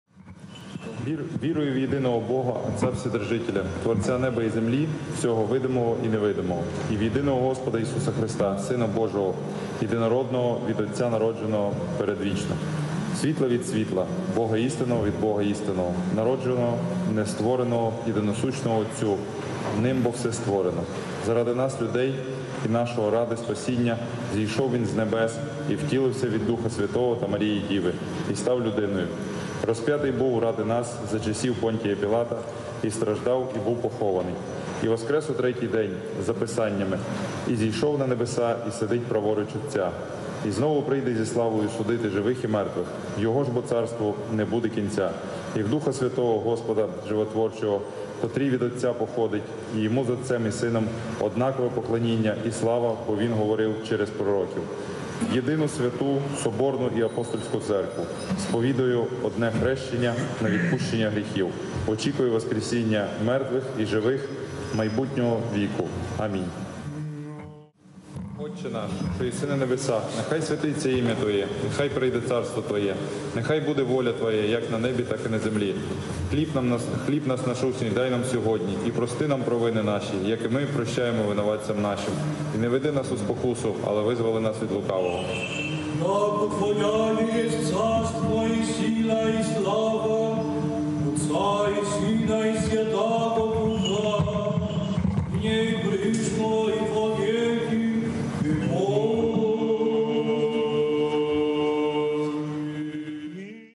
The Prime Minister of Ukraine at the Ecumenical Patriarchate (November 30, 2020)
The Prime Minister of Ukraine Denys Shmyhal says the Credo and the Lord's Prayer during the Patriarchal Divine Liturgy at the Ecumenical Patriarchate, on November 30, 2020.

Ο Πρωθυπουργός της Ουκρανίας στο Οικουμενικό Πατριαρχείο (30 Νοεμβρίου 2020)

Ο Πρωθυπουργος της Ουκρανίας Ντενίς Σμιγκάλ λέει το Σύμβολο της Πίστεως και την Κυριακή Προσευχή κατά την Πατριαρχική Θεία Λειτουργία στο Οικουμενικό Πατριαρχείο, στις 30 Νοεμβρίου 2020.